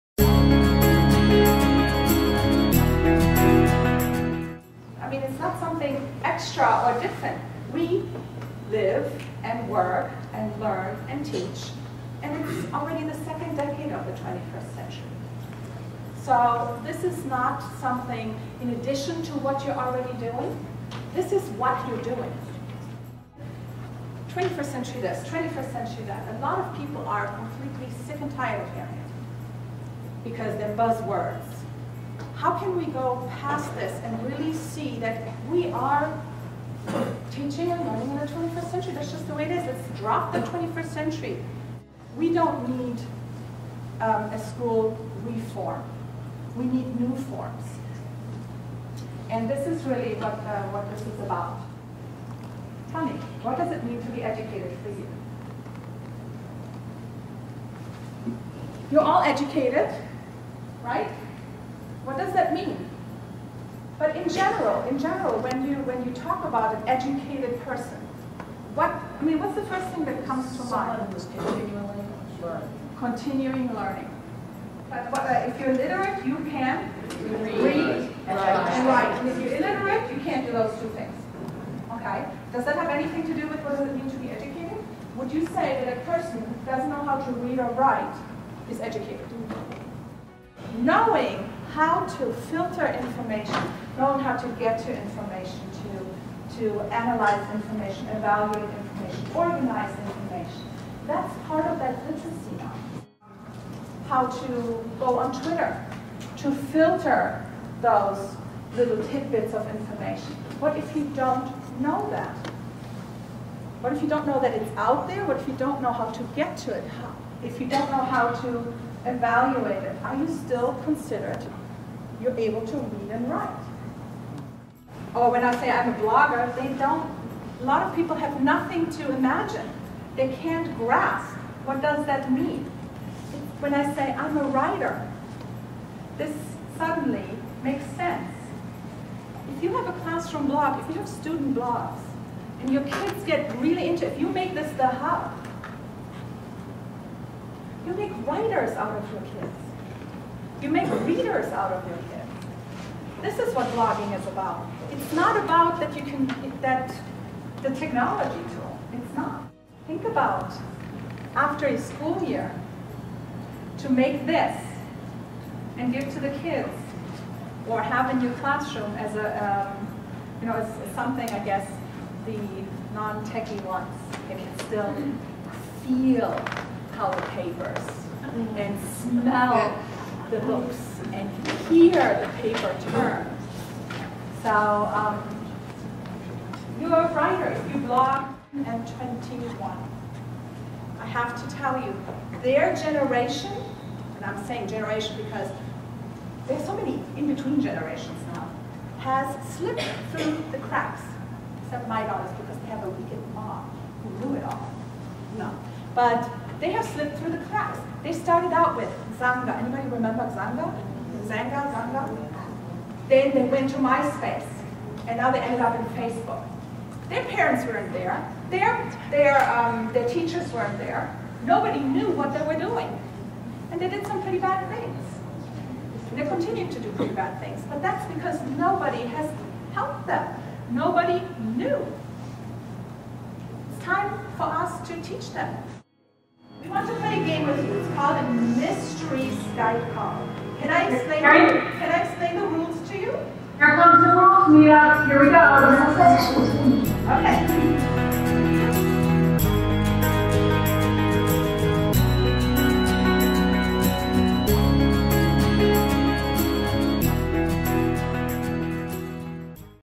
EdWeek SJSD Day 3: Silvia Tolisano
A collection of highlights from Silvia Tolisano's session during day three of EdWeek SJSD. Filmed with a Canon Vixia HD camera and edited with iMovie.